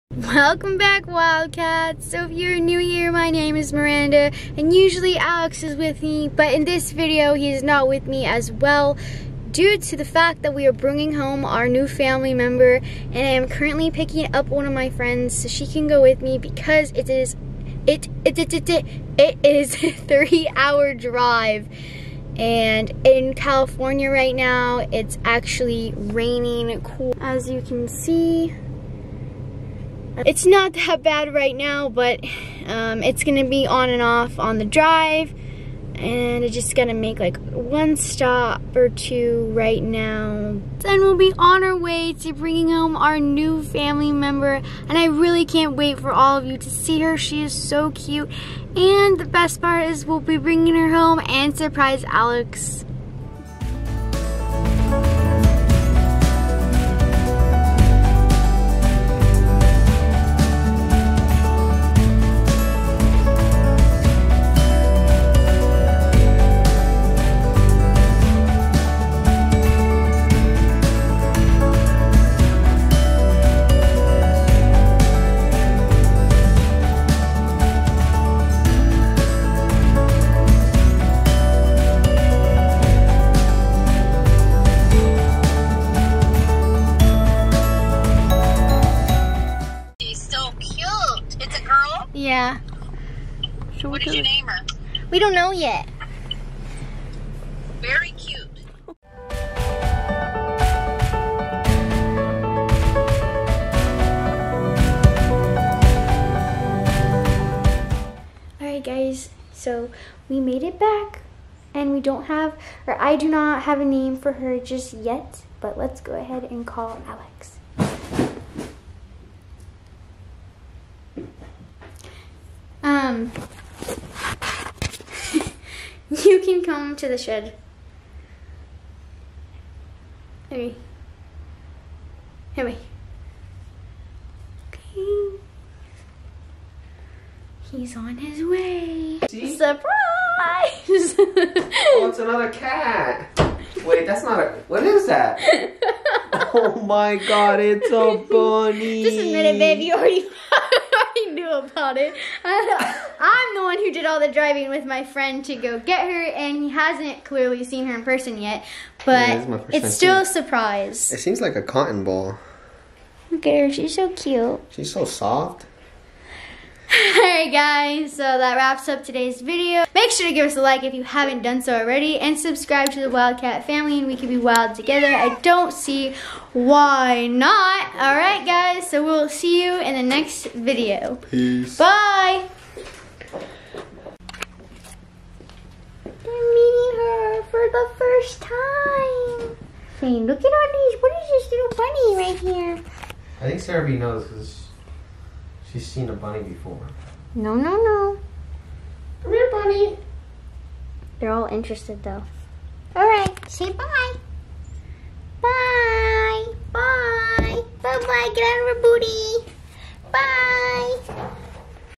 MEET OUR NEW FAMILY MEMBER!
HEY WILD CATS HOWS IT GOING?!
Don't worry we aren't licensed to have a zoo just yet !
New add on to our intro, now that we have a bunny ! Can you guess what it is now ? Stick around for our next video to find out.. SOON we will have a video intro :)
Personal Instagram: a_90s_peachybabe
Cats names :
Sara - b
Sapphire
Hope
Binx
Howlite - Angel

Dogo: Delilah
Bunny: Hazel